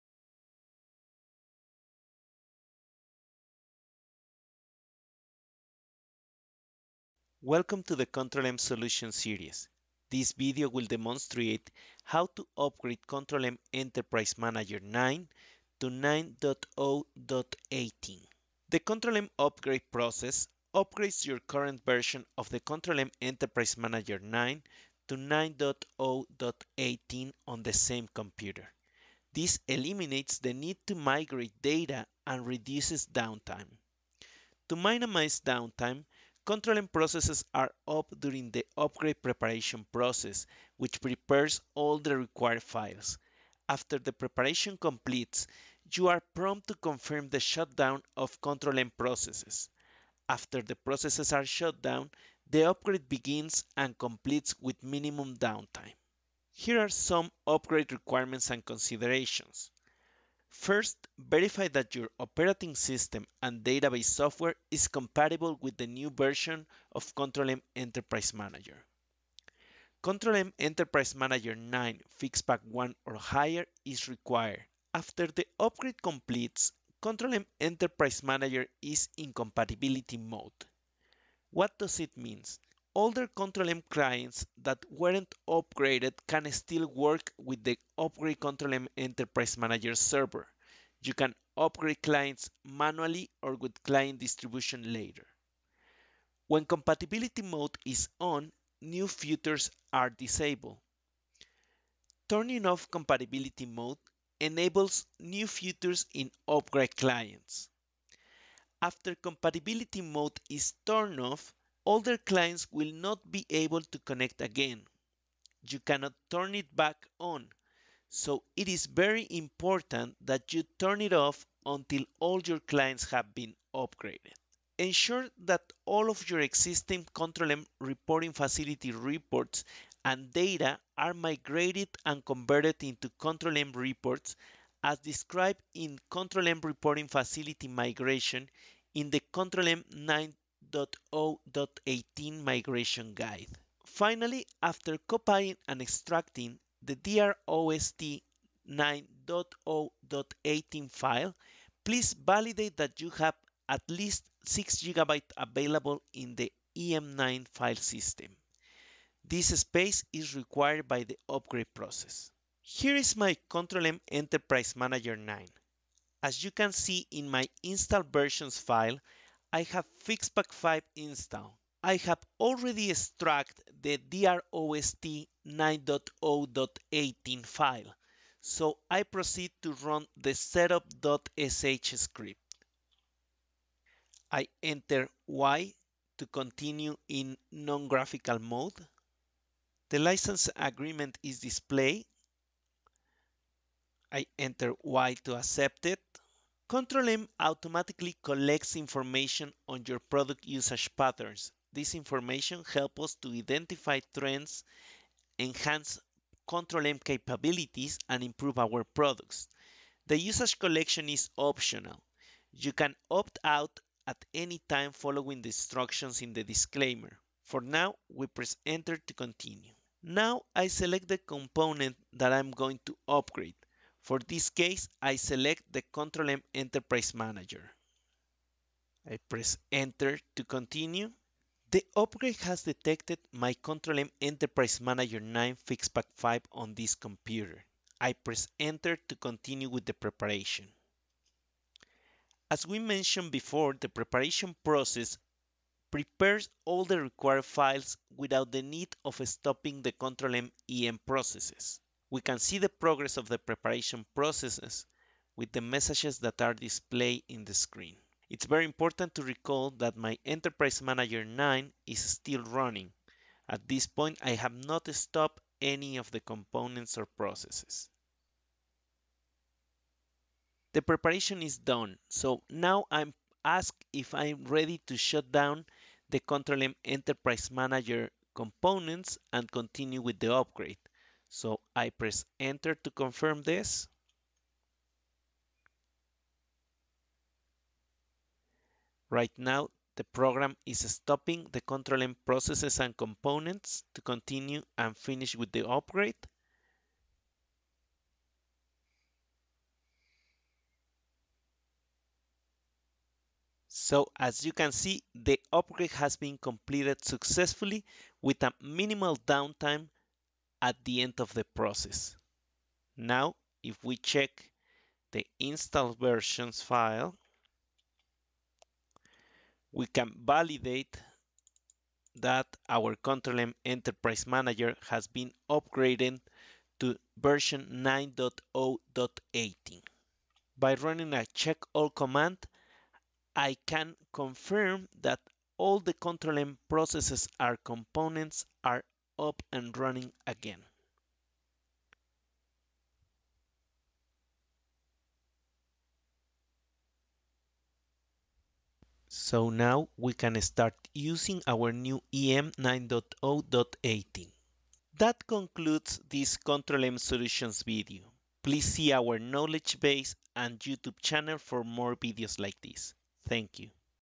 In-place upgrade of Control-M/Enterprise Manager 9.0.00 to 9.0.18
This video demonstrates the process of an in-place upgrade of Control-M/Enterprise Manager 9.0.00 to 9.0.18